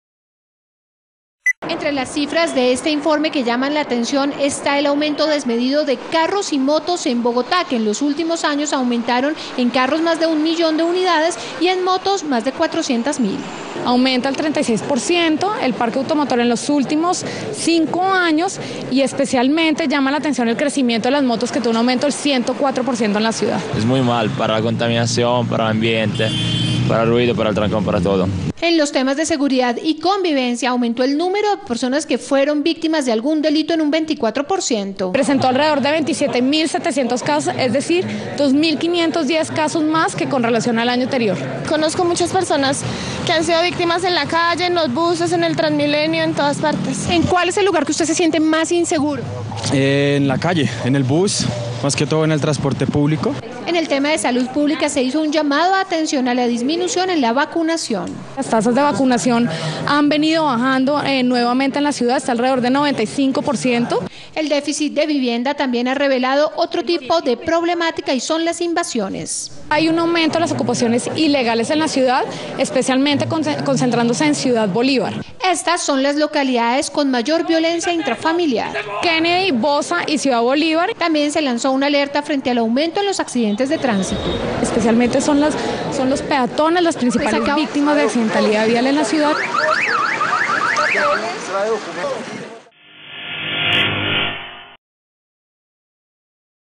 Citytv: ¿Cómo vamos en Bogotá?
Informe revela que ha aumentado en un 36 % el parque automotor y en 104 % el parque de motos en Bogotá.

Emisión: 29 de julio del 2015